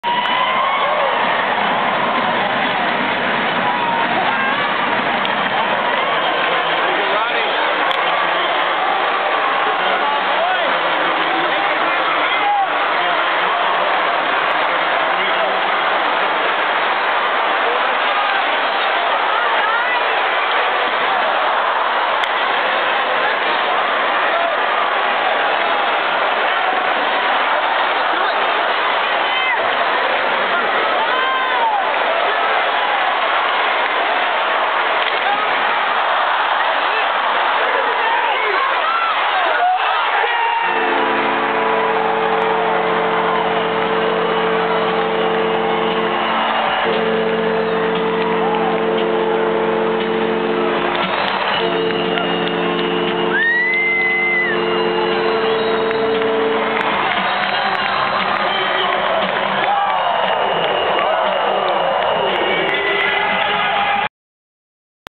Carolina Hurricanes vs. Toronto Maple Leafs 11/02/2008
Ray Whitney blasts a rebound shot in on Toronto's Joseph. This was the beginning of a 3-goal run in 3.5 minutes that gave Carolina the advantage they needed to win the game. Chad LaRose scored the game winner and Eric Staal had a empty netter to seal the deal. Great Game!